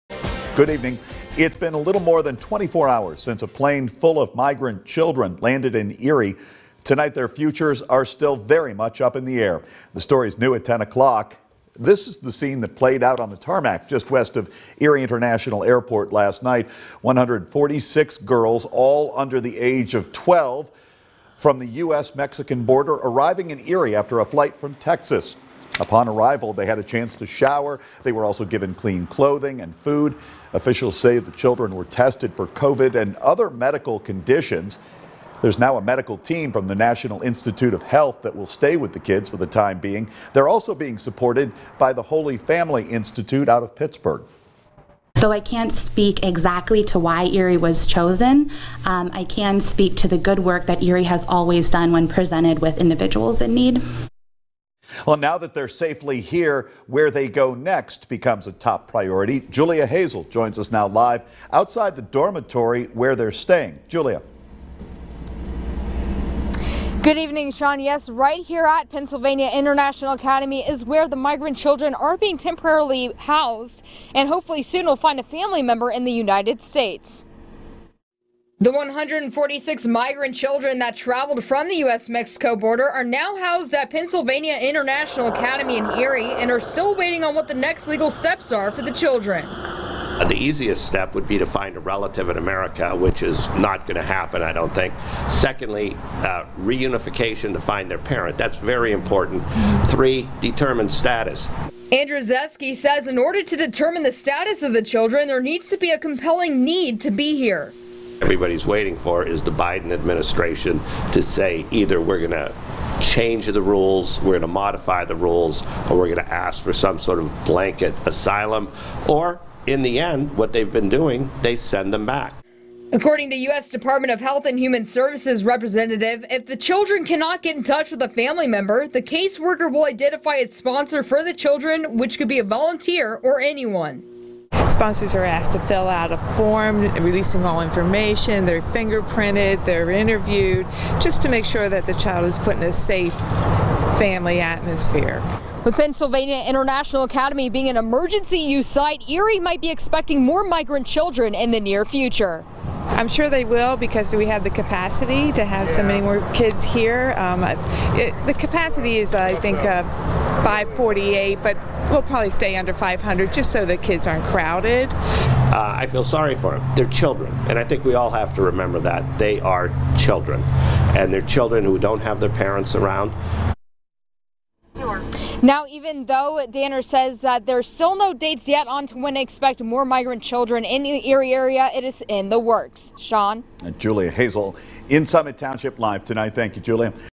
The legal process for the migrant children who arrived in Erie on Tuesday night
Julia Hazel reports